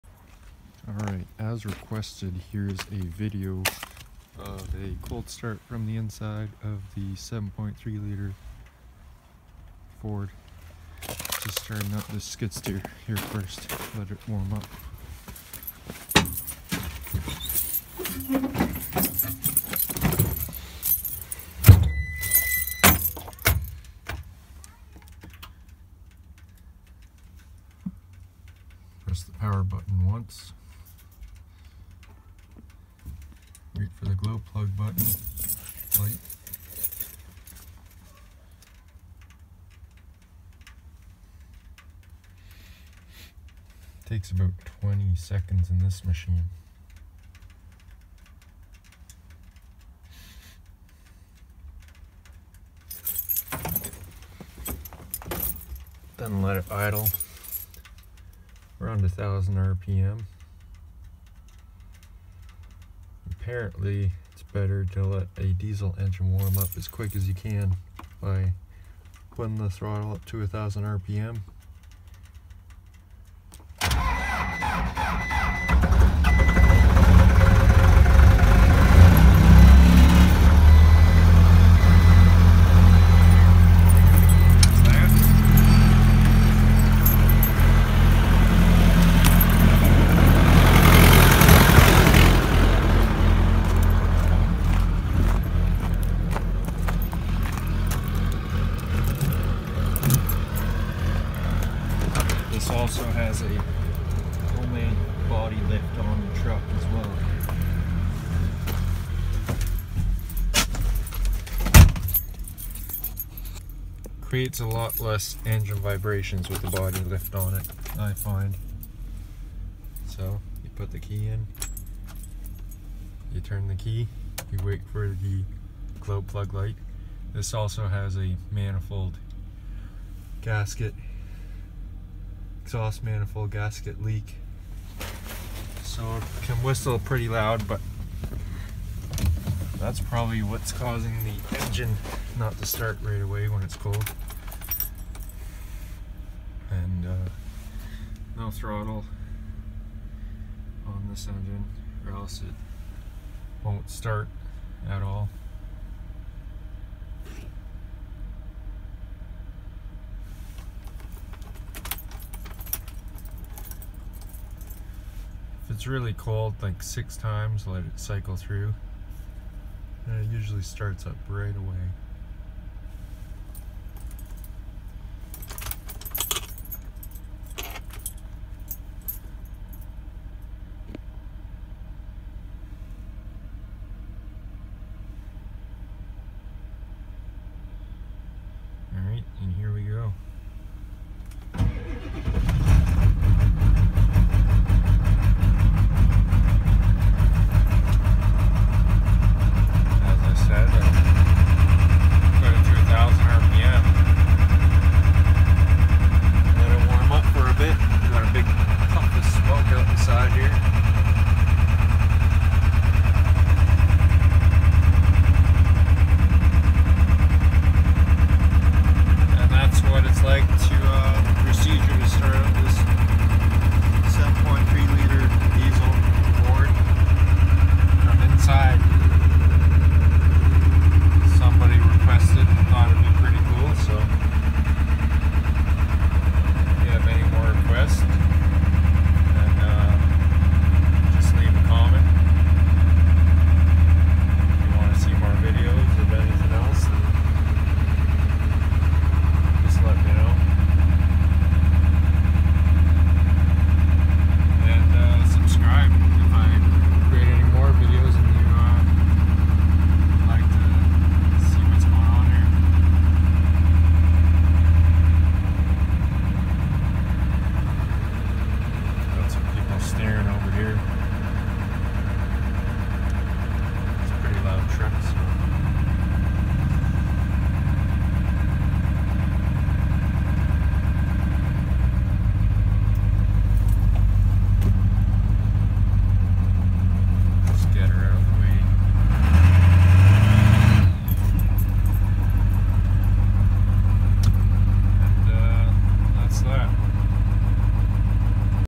Ford diesel cold start in cab
As requested by a viewer who commented on a previous video. 7.3 1995 ford power stroke diesel cold start in cab view of procedure to start.